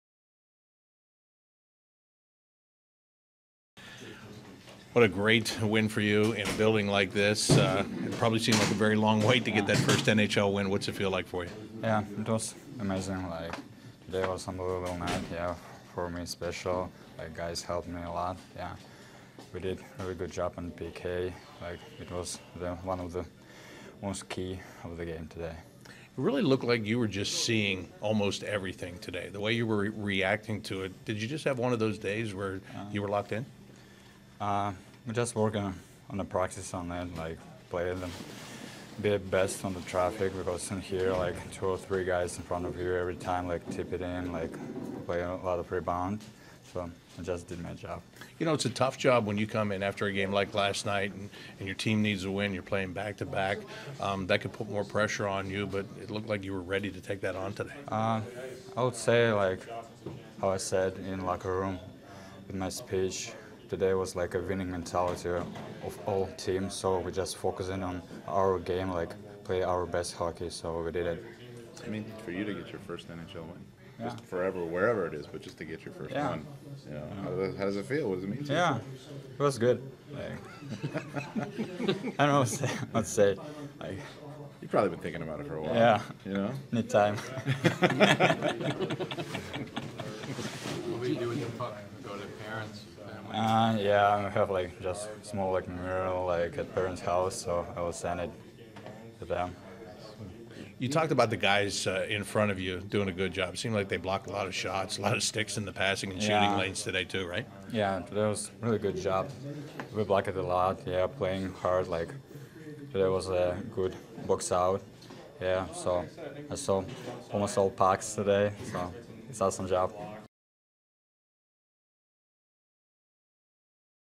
FIRST NHL WIN FOR DANIIL TARASOV ☝️
Daniil Tarasov speaks to the media following his first NHL win - a 5-1 victory over the New York Rangers on Sunday.